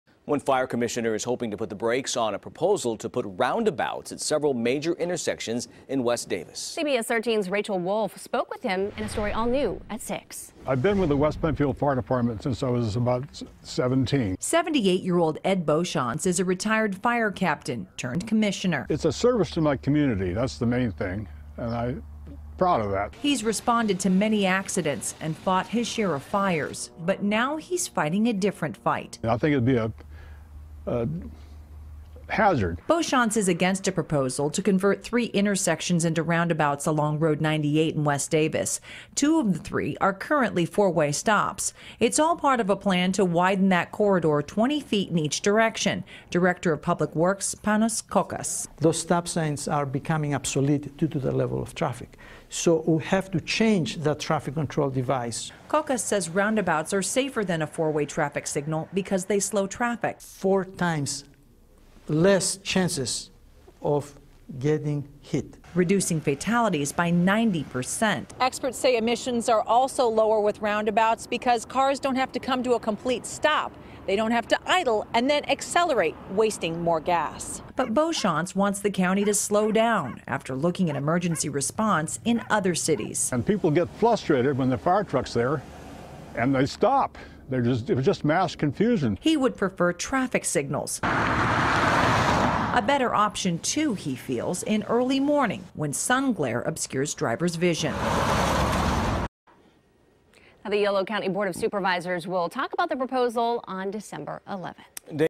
Some Residents Question Proposed Roundabouts In West Davis
A fire commissioner is setting his sights on road work as Yolo County supervisors consider a measure to bring new roundabouts to West Davis.